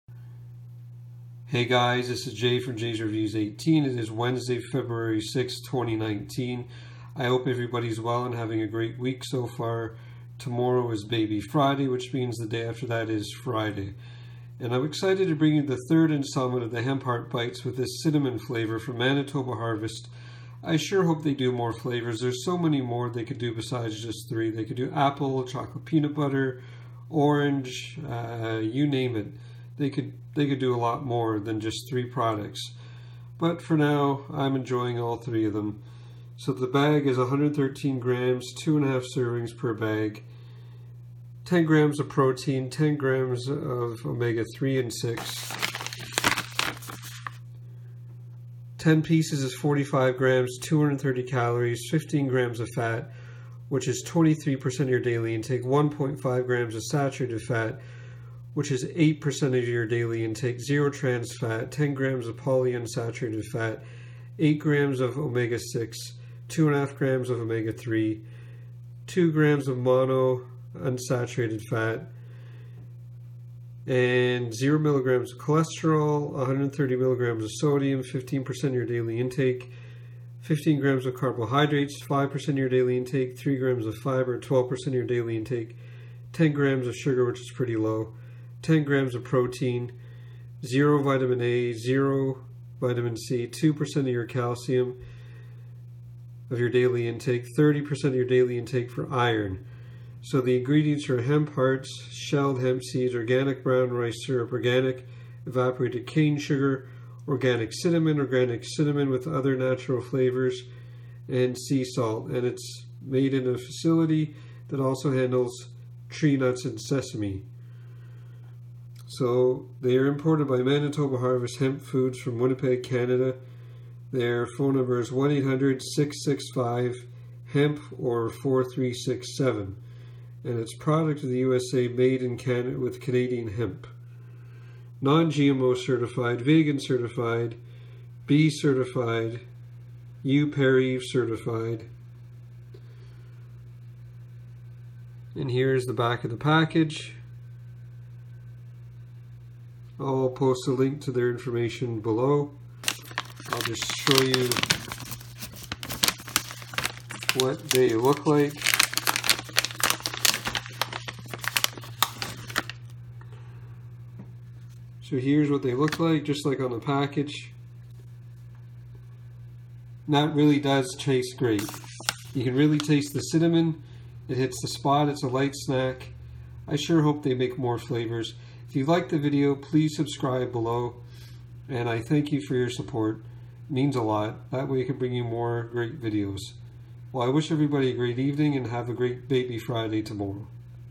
Manitoba Harvest Cinnamon Hemp Bites
Hey guys I'm back with the third and final installment of Manitoba Harvest Cinnamon Hemp bites.  I will show you all the nutritional breakdown in the video.  These bites are definitely a blast of cinnamon making a party in your mouth LOL!

I sure wish they would make more flavours.  There is so much they could do with this product like chocolate peanut butter, almond butter, apple cinnamon and so on.  The only thing I wish is that they would make a bigger bag like before and a bit cheaper.

$6.50 for a small bag gets expensive.  I have a hard time understanding why a bag of candy is far cheaper than this?

All in all I still give this a 5 star review!